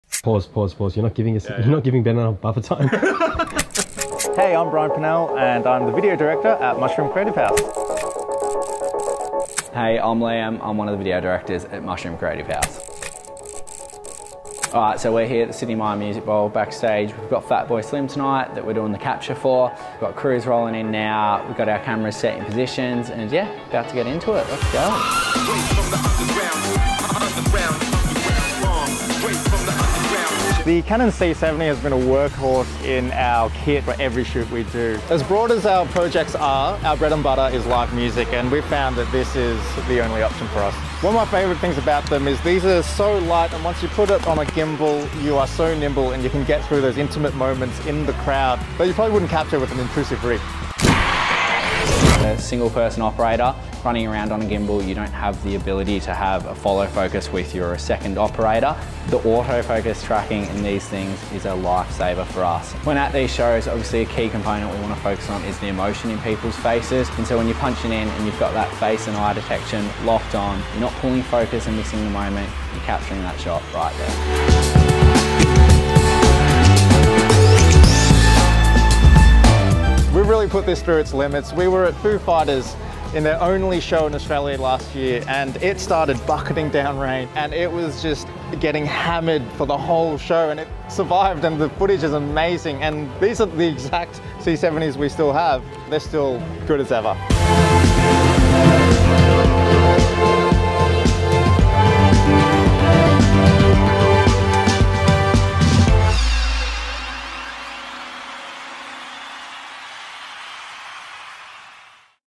Behind the scenes of a Fatboy Slim concert with the EOS C70
Go behind the scenes at the Fatboy Slim concert at the Sidney Myer Music Bowl with Brian Purnell and Liam Pethick, video directors at Mushroom Creative House as they execute their ambitious concert capture with the EOS C70.

To capture the energy and dynamism of live concerts, you need portability, reliability and performance. The EOS C70 is a powerful workhorse that delivers every time, rain, hail or shine.

“One of my favourite things about them is they are so light and once you put it on a gimbal you are so nimble, and you can get through those intimate moments in the crowd that you probably wouldn’t capture with an intrusive rig. As a single person operator, running around on a gimbal you don’t have the ability to have a follow-focus with you or a second operator - the auto focus tracking in these things is a lifesaver for us.”